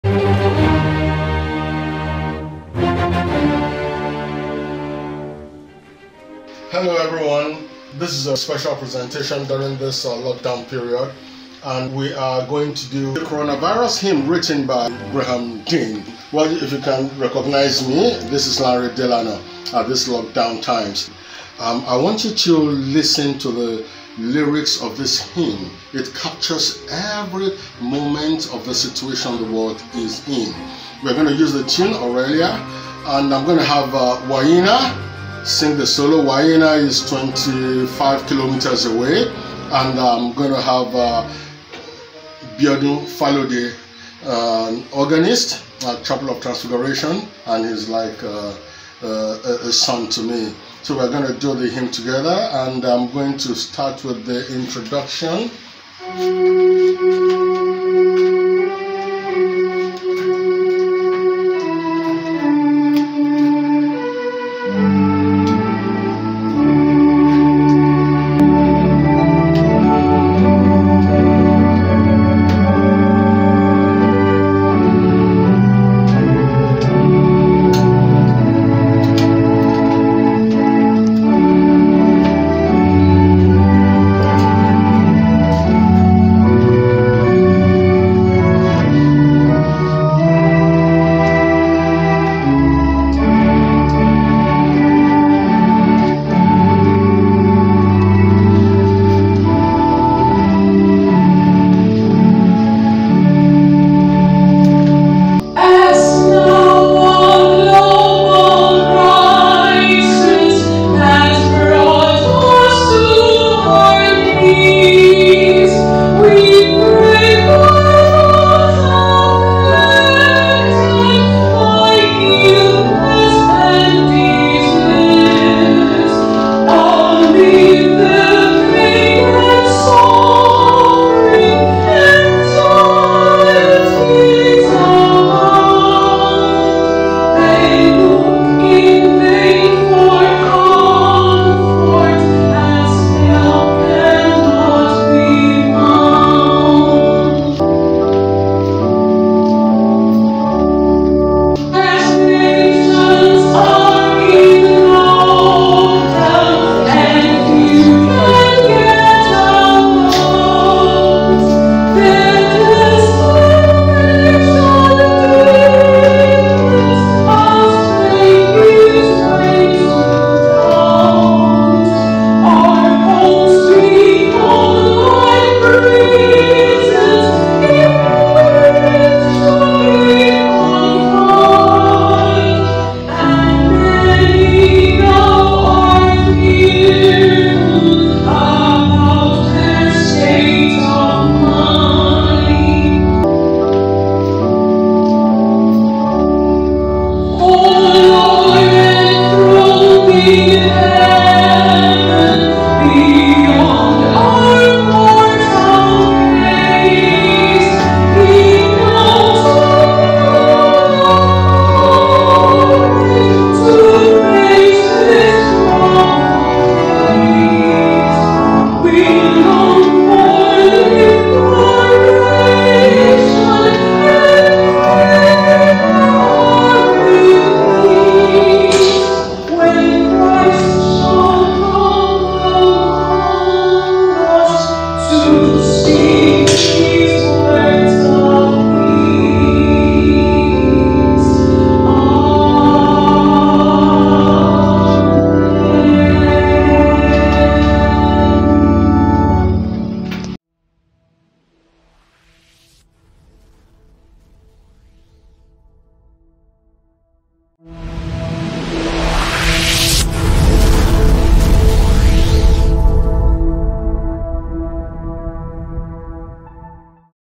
Episode 1 - A CHOPiN Coronavirus Hymn Presentation
Sunday Hymn - Series 1
Credits:
Chopin
Solo Singer - Wanyinna
Organist - Biodun Falode
0:00 - Intro